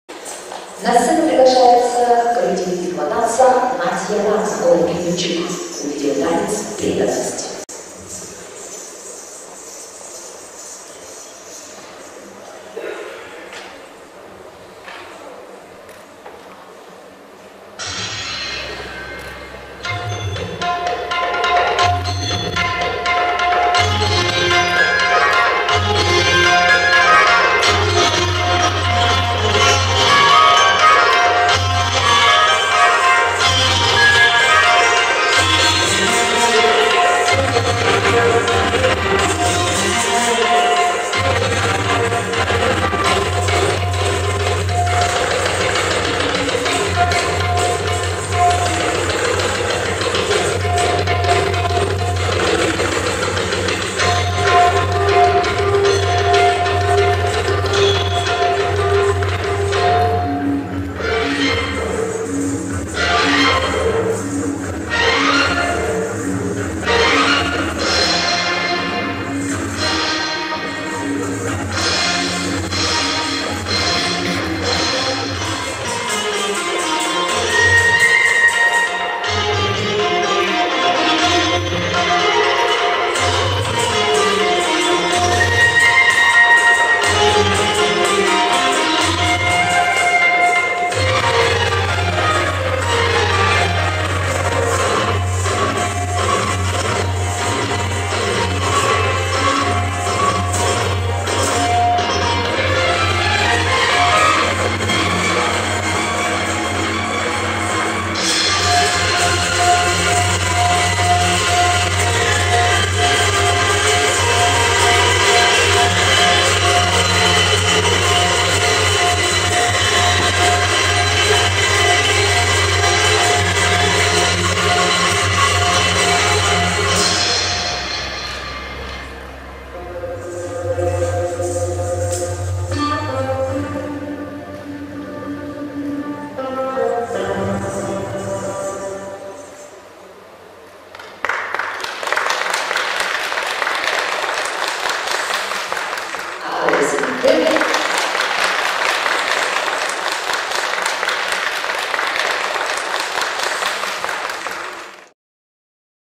Стилизация индийского классического танца катхак "Бхакти нритья"(Танец преданности)
VIII Международный конкурс индийского танца "Bole Chudiyan" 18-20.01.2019
Студия индийского танца "Натья Ратна", рук. Наталья Оноприенко
хореография Сергей Журавлев